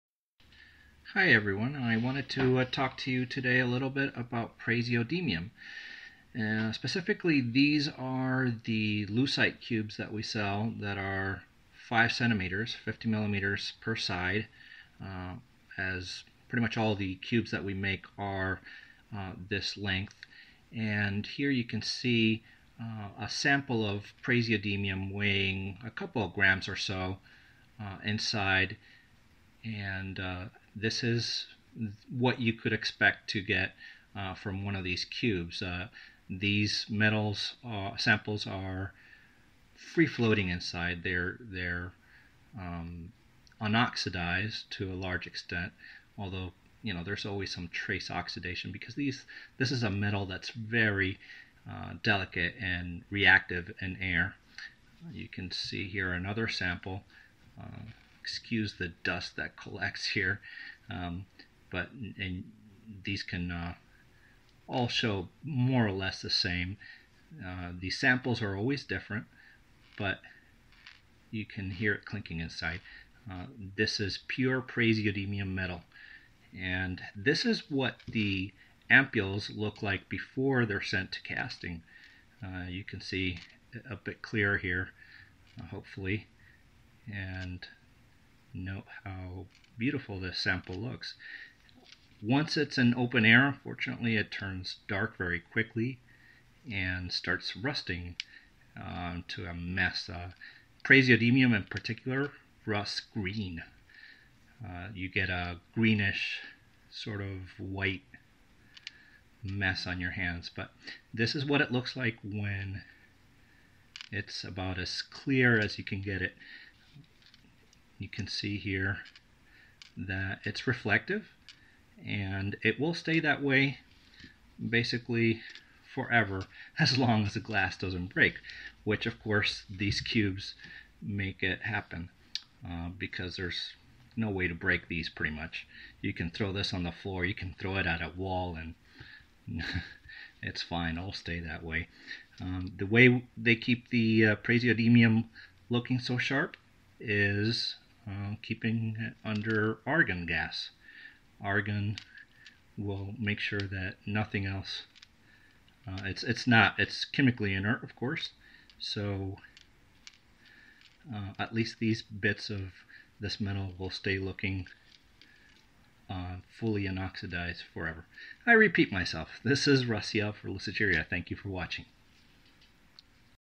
Praseodymium metal in Lucite acrylic cube
Pure praseodymium metal inside glass ampules with and without Lucite acrylic resin casting
Here we have precious, praiseworthy praseodymium (chuckle! snort!)

This rare earth element is a good contender for most exotic sounding name on the periodic table. It is, as comparisons go, one of the easier lanthanides to acquire as it makes up a good portion of the bulk in the monazite and bastnaesite ores from which it's refined. Element 59 is reactive in air and quickly goes from shiny metal to pile of dust. For this reason we always have it stored in ampules under argon. Our samples weigh approximately 5 grams and have been refined to an impressive 99.99% purity.